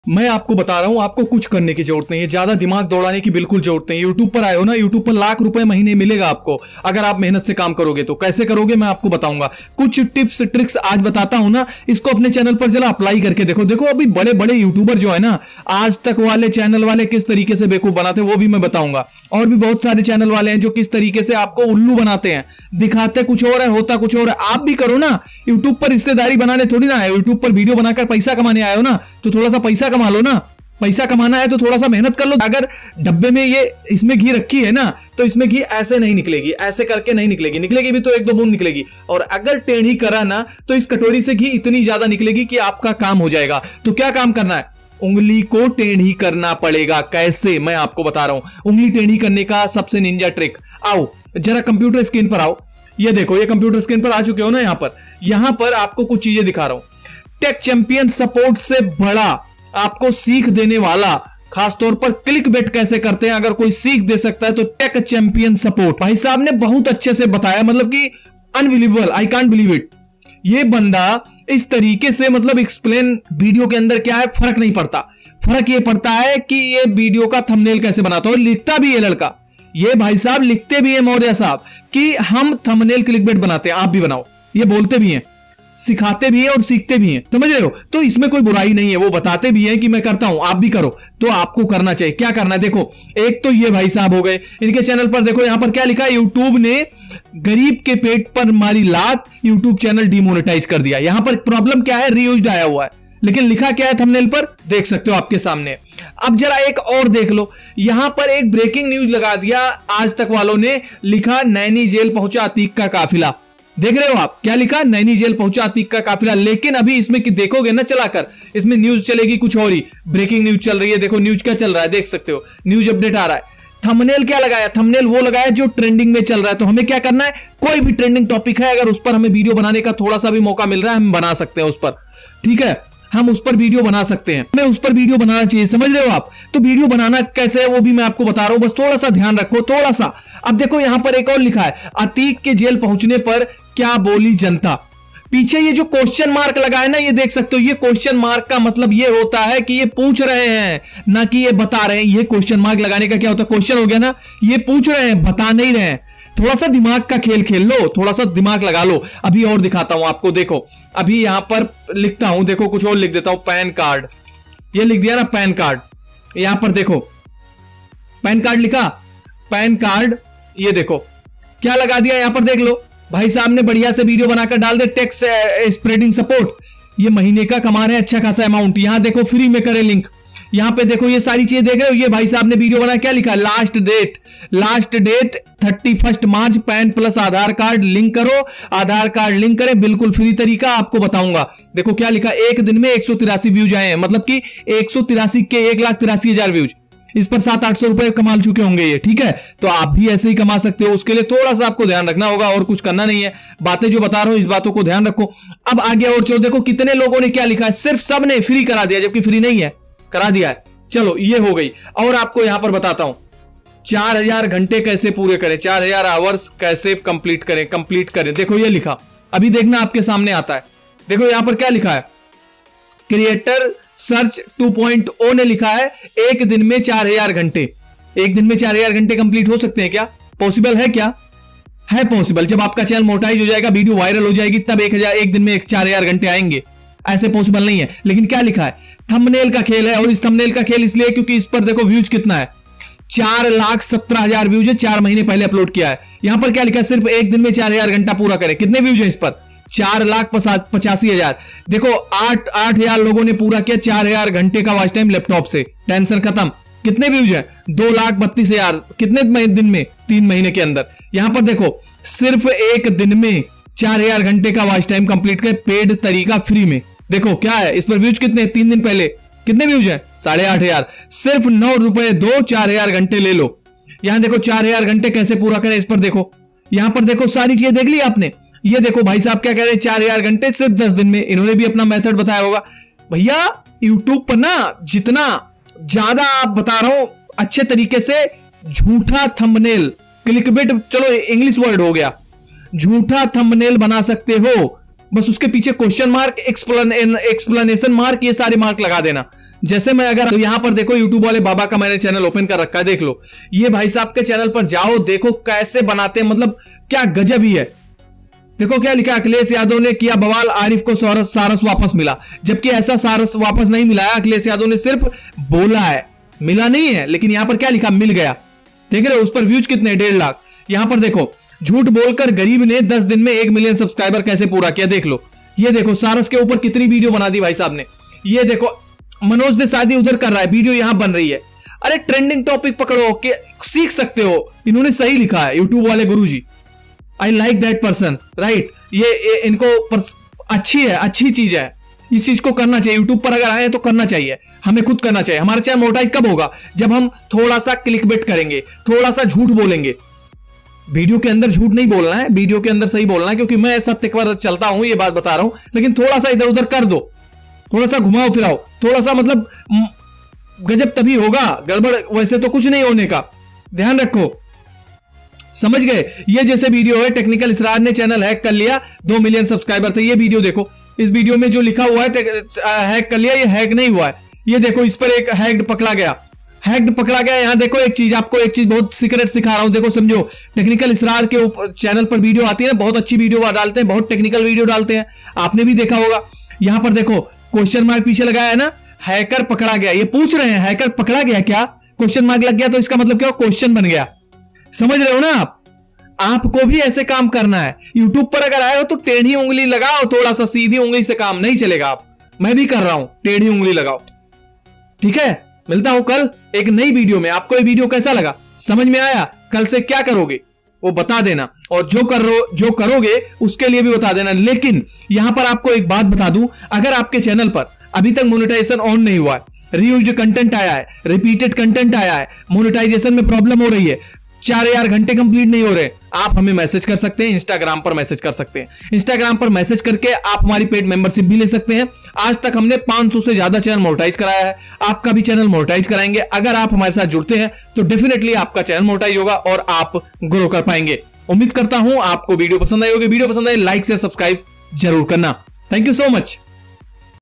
ये Trick Use करना सिख लो || चैनल मोनेटाइज भी होगा और Reused भी नहीं आएगा || 100% गारंटी के साथ
Topics Cover  :-